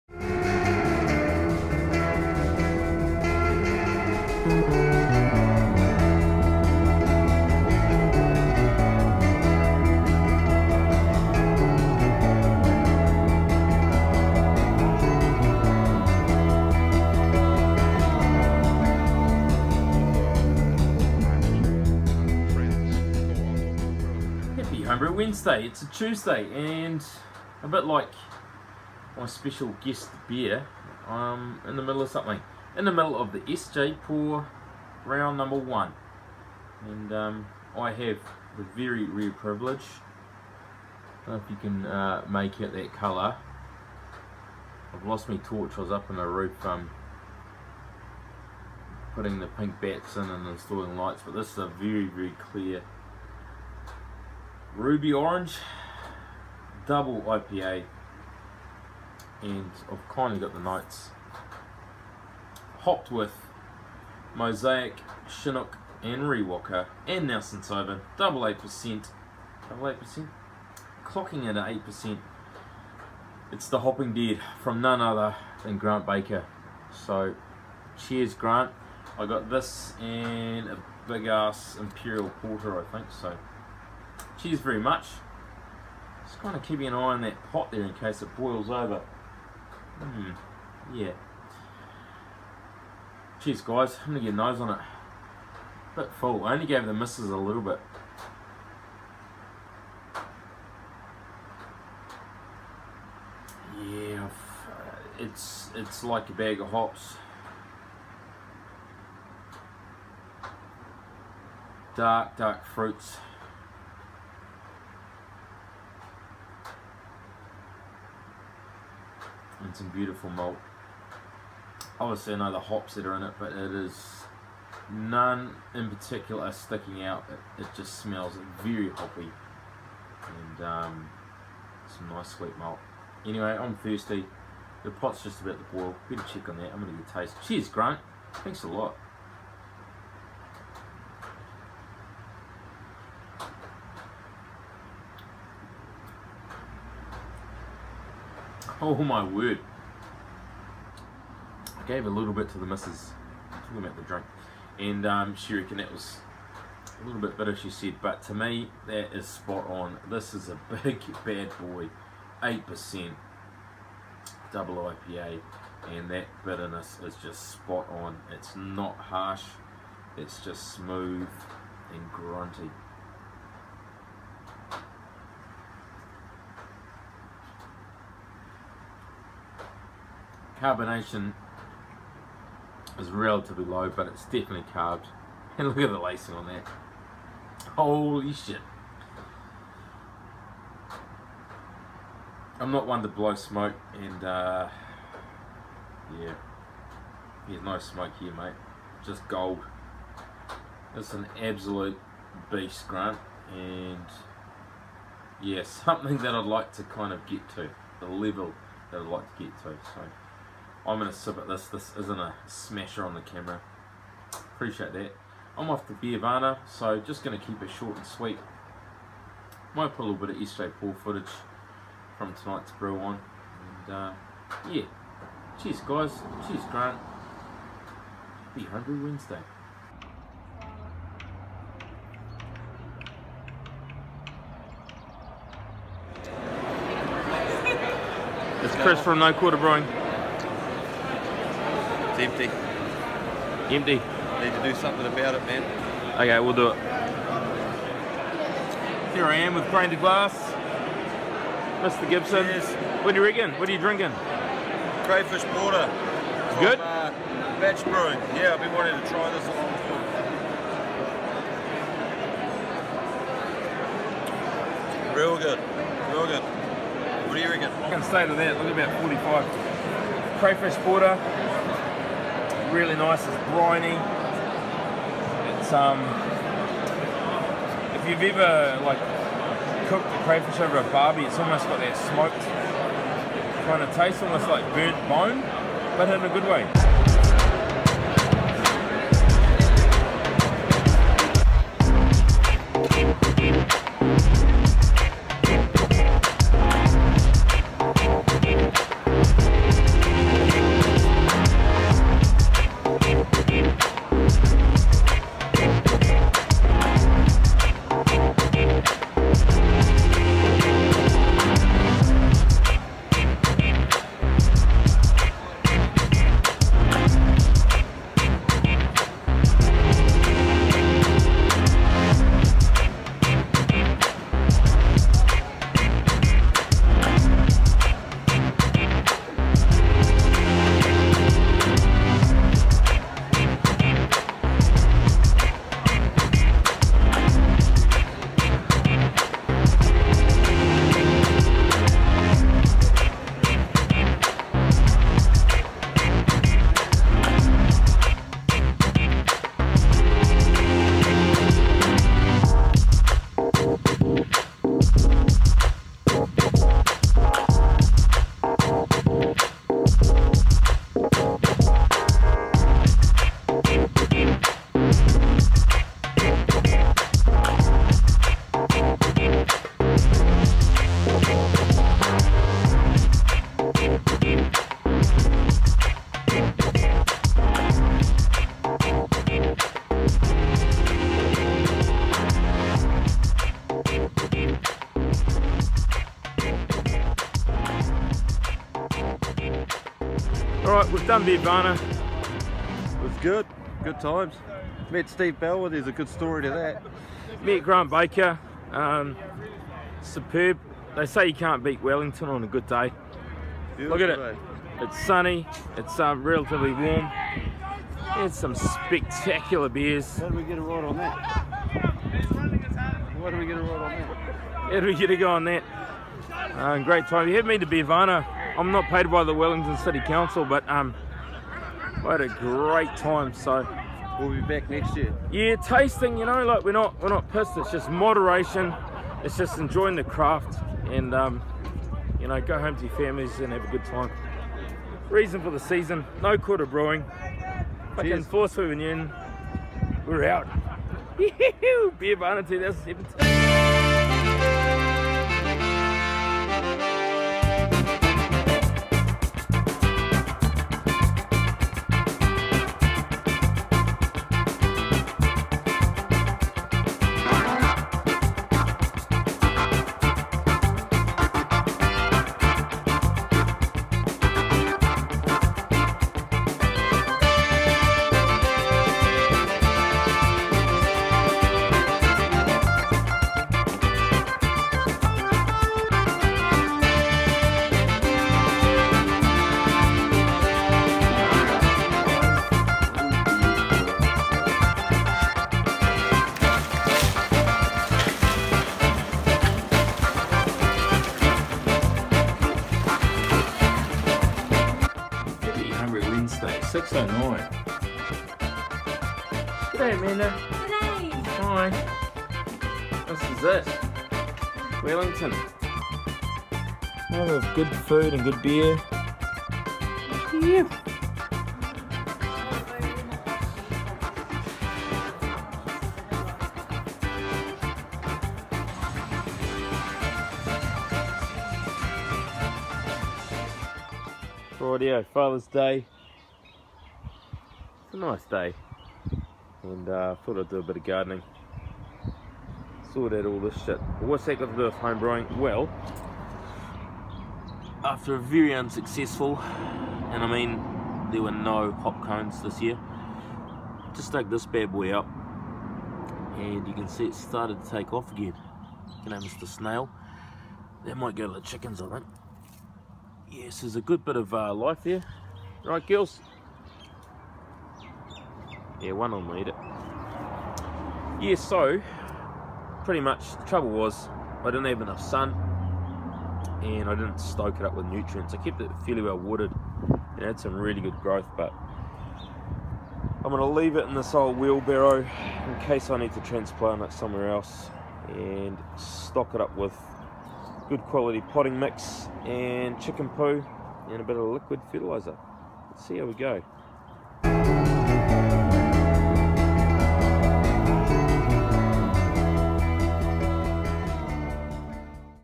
HBW62: Wellington Special: Beervana, Grant Baker's IIPA & Bit's 'n' Pieces
2 back-to-back trips to Wellington, hop planting and a "hopping dead" IIPA from Mr Baker